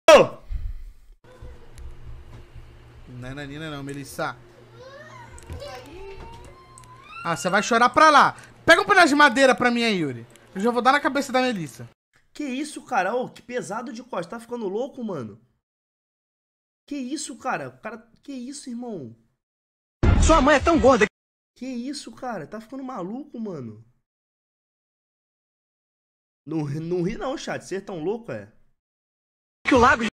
LUQUET4 REAGINDO DCOSTA FALANDO COM A FILHA! *PEGOU PESADO?*
Streamer: Luquet4